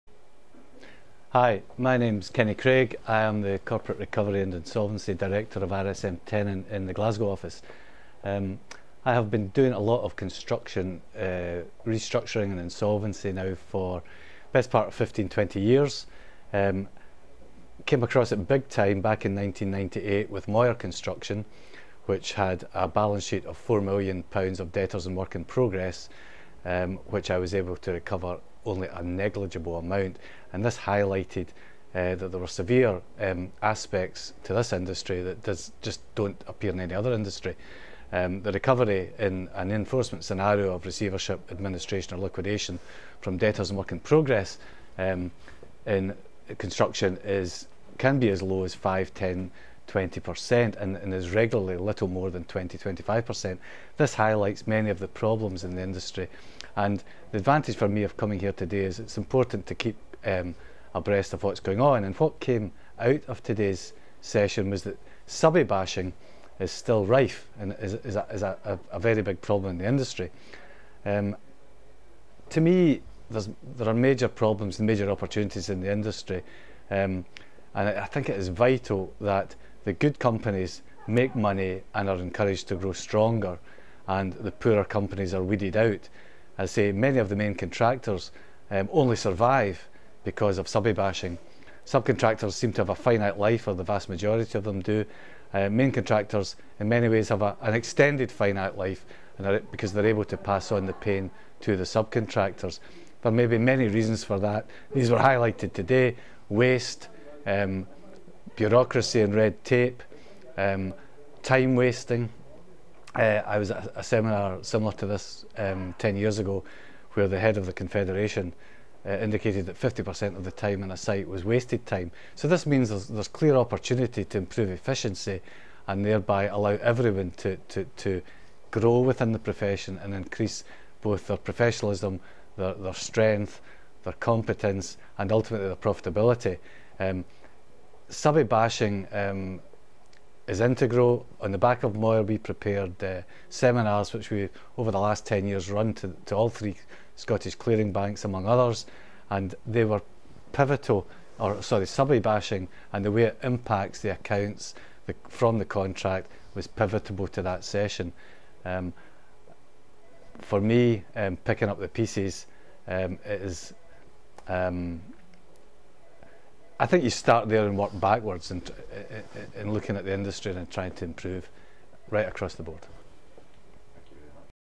Kenny Craig
Kenny Craig one of the delegates gives his views about the Glasgow Constuction Forum on 27. April 2012